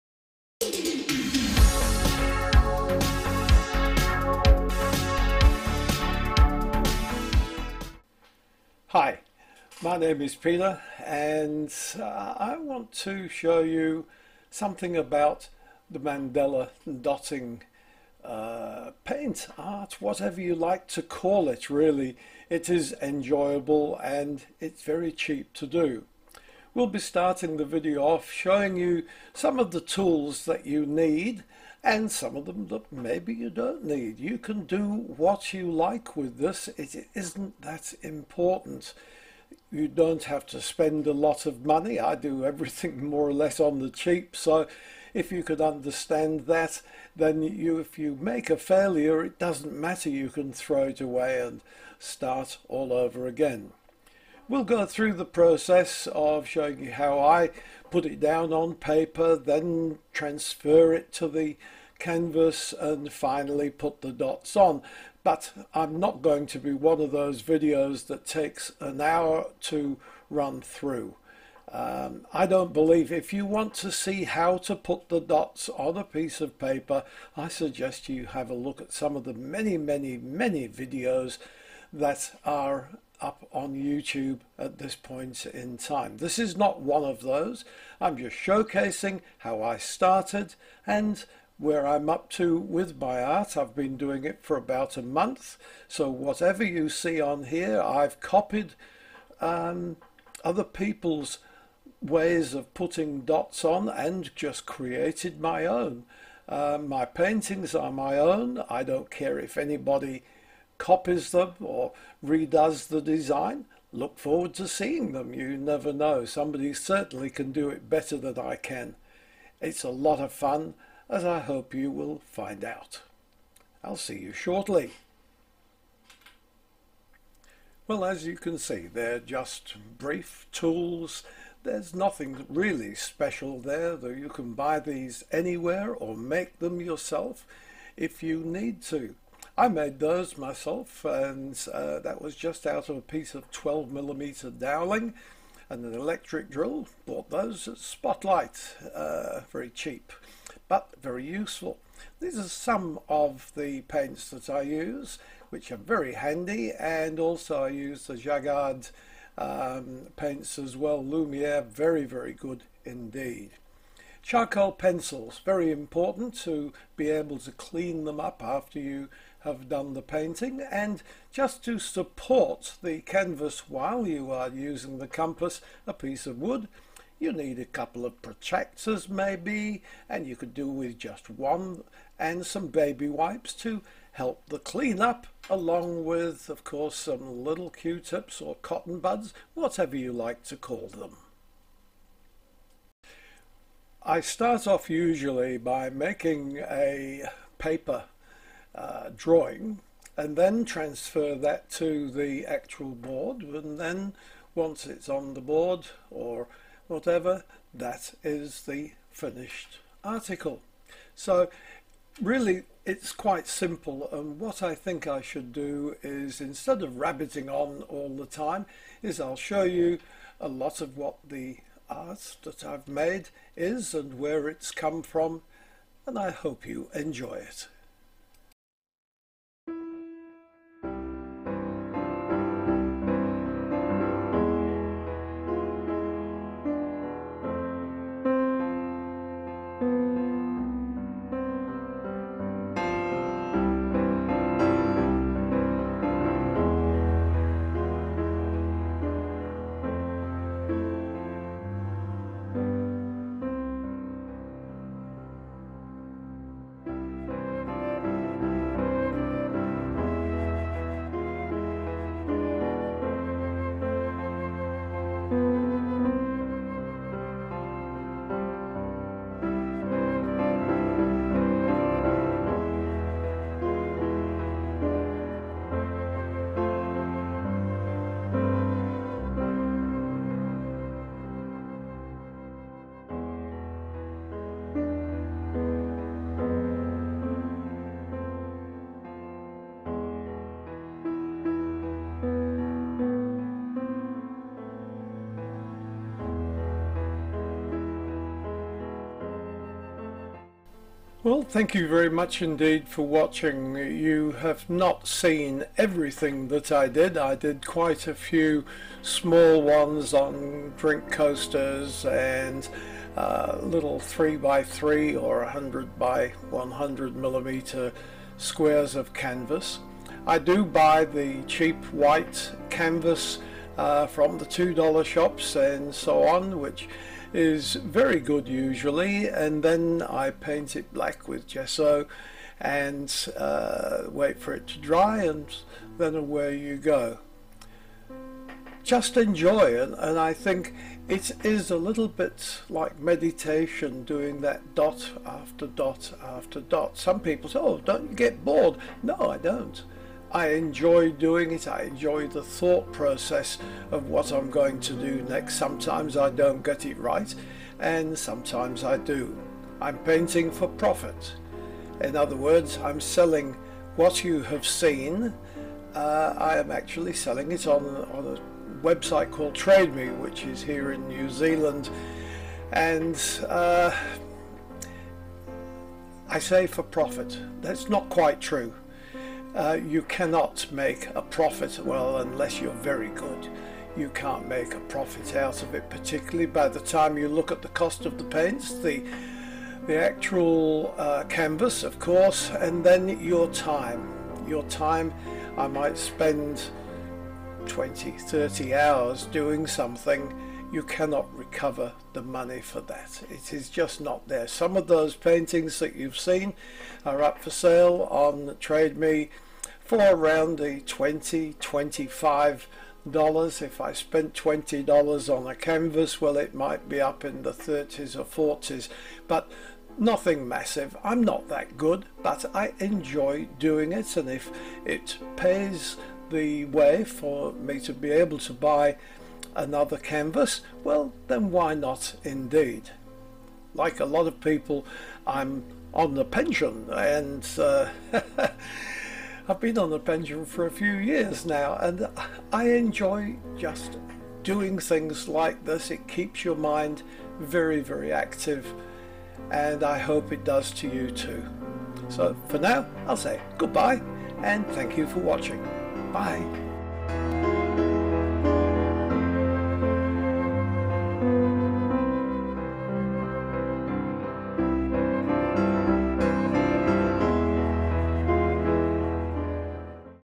Mandala Dot Art...Is it for you?????????
It must be I feel one of the simplest forms of art, but Is It For You???
I havent got that gene you have for being able to draw pretty pictures. but once I found I could do it using dots it became clear that not only was it relaxing but quite pretty as well.
I am happy to help anyone in any way I can so dont be afraid to ask questions